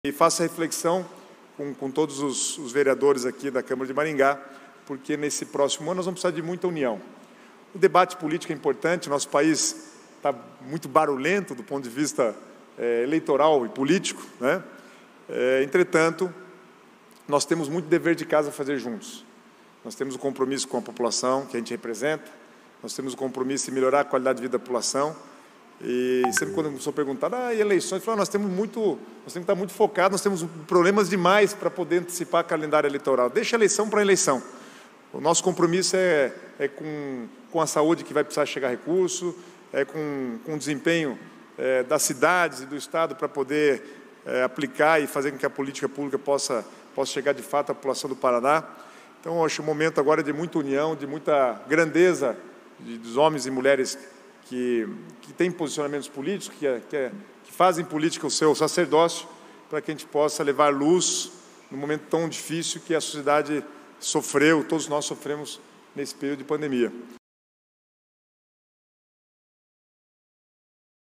GUTO SILVA diz que "é preciso deixar as eleições para o momento das eleições"
Convidado pelo vereador Alex Chaves (MDB), o Chefe da Casa Civil Guto Silva chegou à Câmara por volta das 10h30 da manhã desta quinta-feira, 11, e mesmo com um certo atraso em sua agenda, conversou AO VIVO com a equipe de O FATO MARINGÁ WEB TV. Guto disse que a visita era importante porque o Paraná está saindo da pandemia e precisa pensar na retomada da economia. "Temos que passar uma mensagem de otimismo e mostrar os resultados da atuação do governo", disse o Chefe da Casa Civil .

Temos que passar uma mensagem de otimismo e mostrar os resultados da atuação do governo", disse o Chefe da Casa Civil a O FATO

Vozes nos bastidores do Legislativo davam conta de que o verdadeiro objetivo da visita de Silva era eleitoral. O vereador Mário Verri (PT), disse em entrevista a O FATO "que mais do que prestação de contas", via na visita de Guto "uma pré-campanha eleitoral", mas que era um direito desde que não fosse usada a estrutura do governo para isso.

"Mais do que prestação de contas, visita de Guto é pré-campanha", diz Mário Verri (PT)

A participação de Guto Silva na sessão da Câmara Municipal de Maringá terminou com o augurio do presidente Mário Hossokawa ao Chefe da Casa Civil de Ratinho as próximas eleições. "Eu penso que a partir de 2023 pela primeira vez nós vamos teremos um senador maringaense no Senado Federal".

"Eu penso que a partir de 2023 pela primeira vez nós vamos teremos um senador maringaense no Senado Federal", disse o presidente da Câmara

A fala de Mário foi muito aplaudida, mas Guto Silva, que muitos já indicam como futuro candidato do PSD ao Senado Federal, fez questão de dizer que não veio a Maringá para fazer campanha, mas para prestar contas e falar da atuação do Governo do Estado.

Veja o que diz Guto Silva sobre as eleições

"O debate político é importante, mas nós temos muitos deveres de casa para fazermos juntos. Temos compromisso de melhorar a qualidade de vida da população, mas muitos me perguntam e as eleições, e eu digo; Temos problemas demais para podermos antecipar o calendário eleitoral, vamos deixar as eleições para o período das eleições", justificou Guto Silva.

Guto Silva disse na tribuna da Câmara que a população quer saber o que vai acontecer após a pandemia e que agora era o momento de reconstruir tudo o que a pandemia tirou da normalidade. Silva lembrou dos obstáculos que as crianças estão enfrentando no retorno às aulas e que o governo está preocupado com todos os problemas, inclusive de ordem psicológica em relação aos estudantes. O Chefe da Casa Civil ressaltou que os indicadores econômicos indicam que a indústria e o setor de serviços voltarama a crescer e que a grande preocupação está ligada ao aumento da inflação que deteriora a vida das cidades. "A inflação come os salários e deteriora o tecido social", disse Guto.

Sobre a Saúde, Guto do rebote da pandemia. Ele explica que todo o sistema de saúde ficou congelado para atender a pandemia e isso gerou represamento das cirurgias eletivas. "Queremos destravar as cirurgias eletivas e manter recursos na área para atender a população".

Além de Saúde, Educação e Economia que vai ter que conter a inflação, Guto disse que o Governo vai investir muito na área social e que o debate político tem que superar as questões ideológicas, conviver com as diferenças porque só o diálogo poderá levar o país a encontrar soluções para os problemas.